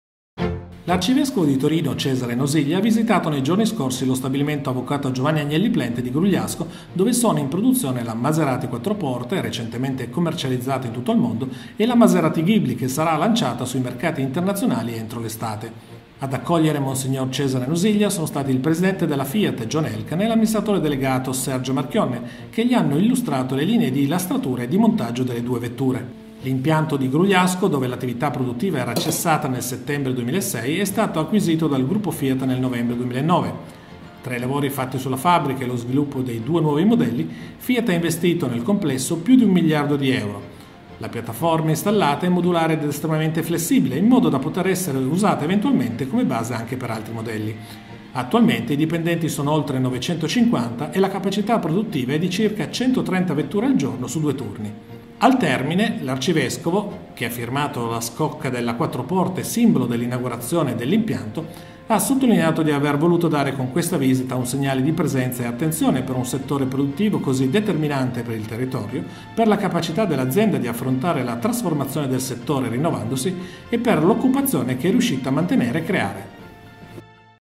MONS. CESARE NOSIGLIA VISITA STABILIMENTO DI GRUGLIASCO (TO)
L'Arcivescovo di Torino, Cesare Nosiglia, ha visitato lo stabilimento "Avv. Giovanni Agnelli Plant" di Grugliasco (Torino)

L'Arcivescovo di Torino, Cesare Nosiglia, ha visitato il 13/6/2013 lo stabilimento "Avv. Giovanni Agnelli Plant" di Grugliasco (Torino), dove sono in produzione la Maserati Quattroporte, recentemente commercializzata in tutto il mondo, e la Maserati Ghibli che sarà lanciata sui mercati internazionali entro l'estate.

Ad accogliere Mons. Cesare Nosiglia sono stati il Presidente della Fiat, John Elkann e l'Amministratore Delegato, Sergio Marchionne.

Nel corso della visita, l'Arcivescovo ha potuto vedere le linee di lastratura e di montaggio delle due vetture. Al termine ha sottolineato che quello di oggi è stato "un segnale di presenza e attenzione che ho voluto dare per un settore produttivo così determinante per il nostro territorio, per la capacità dell'azienda di affrontare la trasformazione del settore rinnovandosi e per l'occupazione che è riuscita a mantenere e creare".

L'impianto di Grugliasco, dove l'attività produttiva era cessata nel settembre 2006, è stato acquisito dal Gruppo Fiat nel novembre 2009.  Tra i lavori fatti sulla fabbrica e lo sviluppo dei due nuovi modelli, Fiat ha investito, nel complesso, più di un miliardo di euro. La piattaforma installata,  secondo i più moderni  concetti e regole del World Class Manufacturing,  è modulare ed estremamente flessibile, in modo da poter essere usata eventualmente come base anche per altri modelli. Attualmente i dipendenti sono oltre 950 e  la capacità produttiva è di circa 130 vetture al giorno su due turni.

Grugliasco (Torino), 13 Giugno 2013